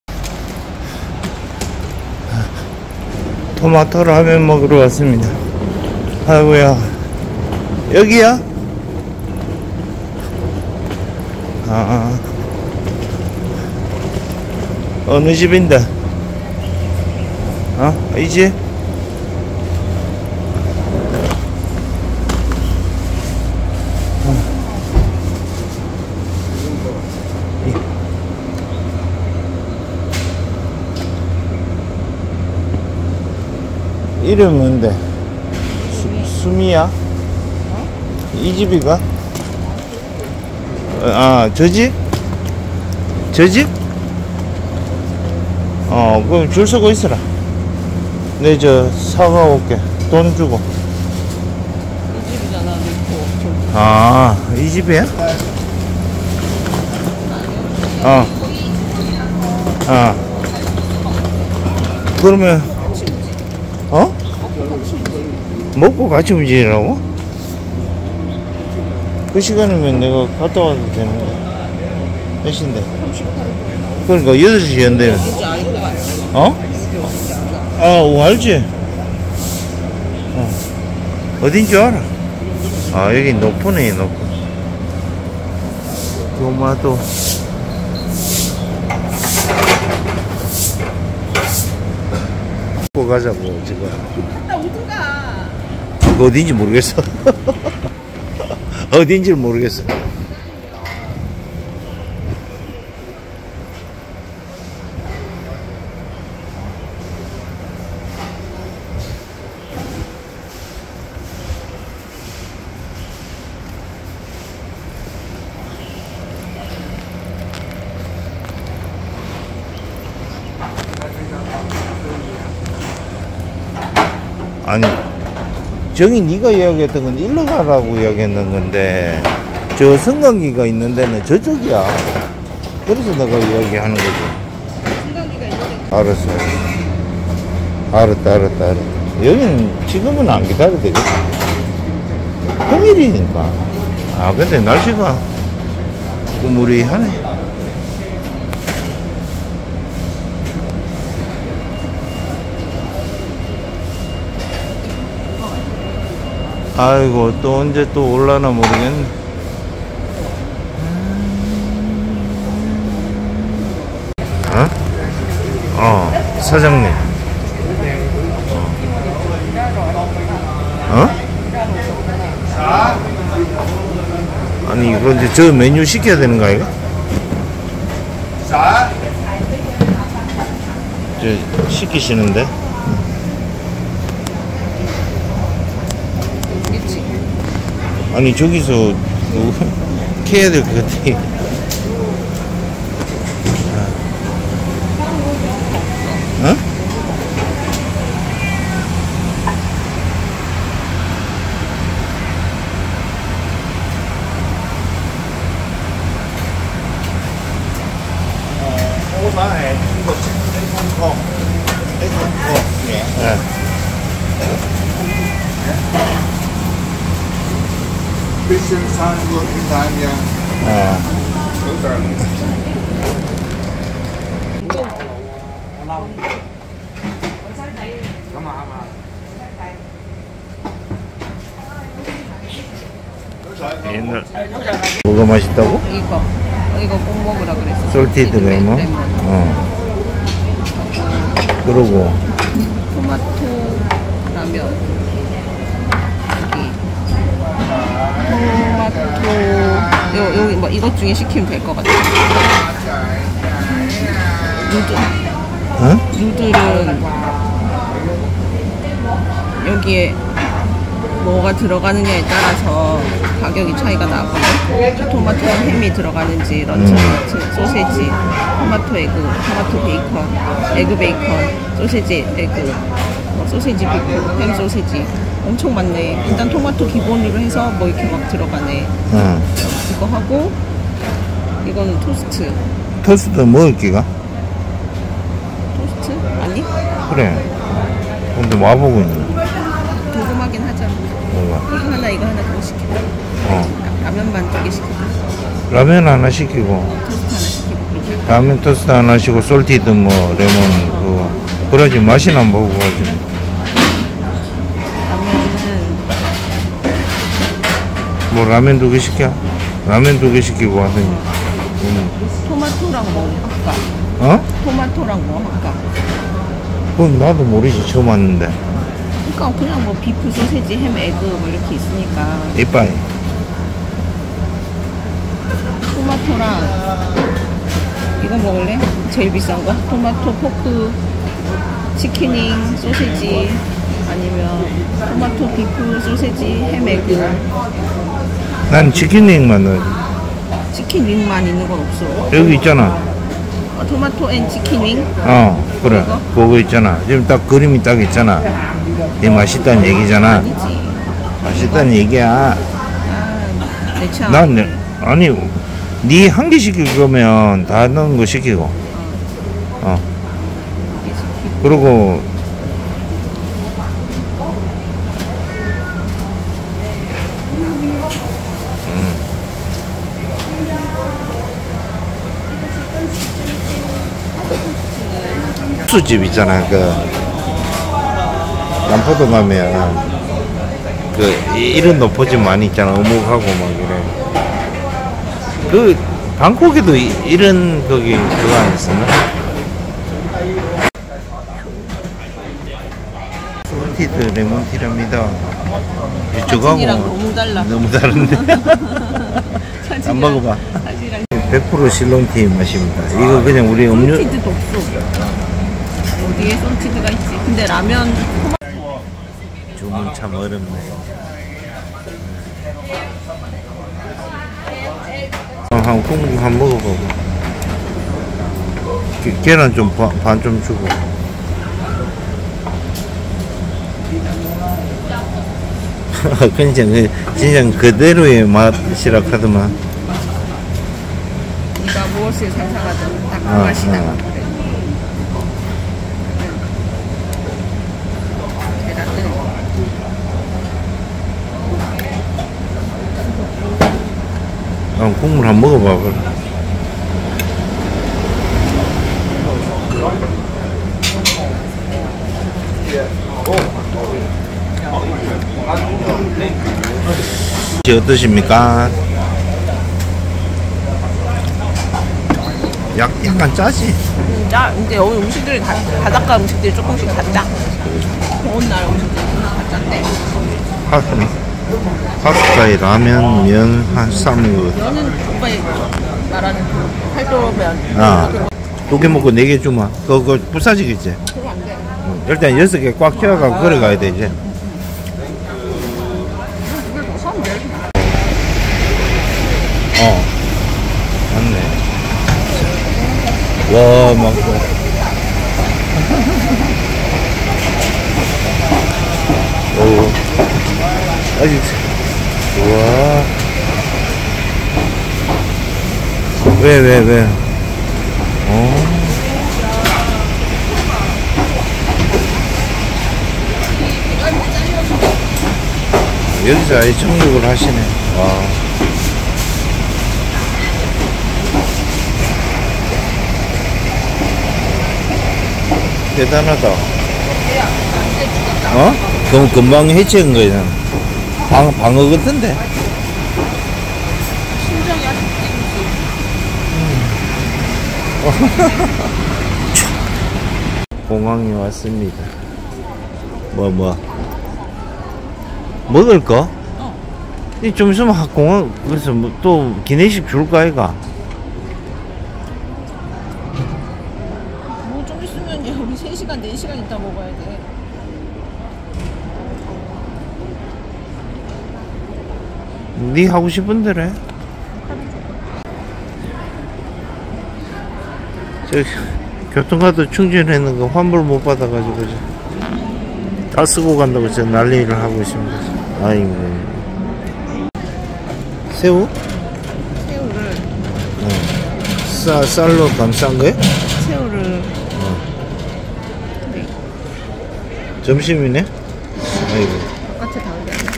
24 홍콩여행 - 마지막날 아침 토마토 라면
홍콩여행 마지막 날이 왔읍니다..  아쉬움이 많이 남지만
코로나 이후 처음 해외 여행이라  기분이 새로워 지네요
토마토 라면이라  특별한 맛이 있는 줄 알지만 있는 그대로 본연의
맛입니다.   한번 맛 볼 수 는 있지만.. 추천은...